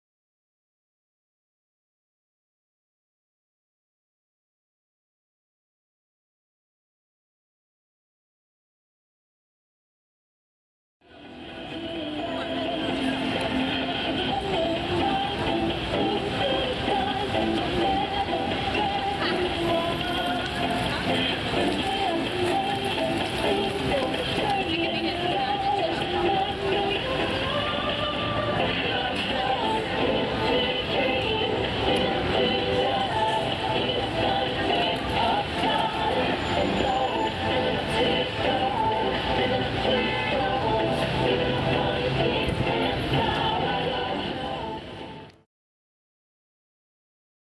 OUR EUROPE TRIP 2009 - THE CHAMPS ELYSEES PAPY DANCER
On a busy bus stop, about 100 metres from the Arc de Triomphe, 47 seconds of entertainment!  See also, "The Champs Elysees Skirmish.